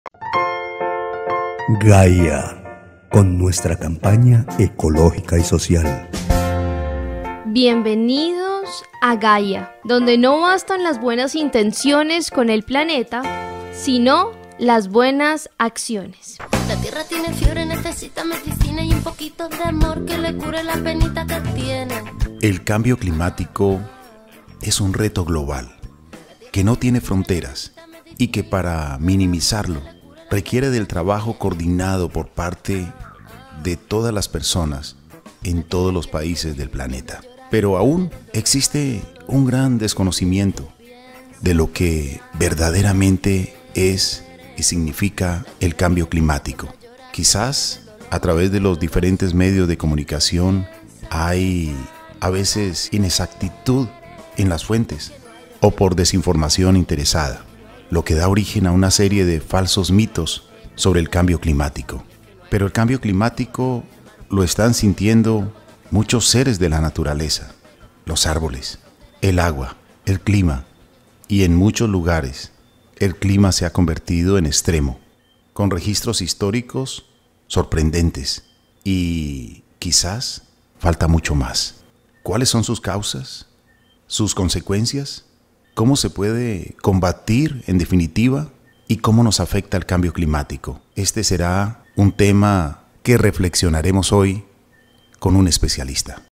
el Cambio Climatico
El cambio climático es un reto global que no tiene fronteras y que para minimizar los efectos requiere del trabajo coordinado por parte de todas las personas en todos los países del planeta.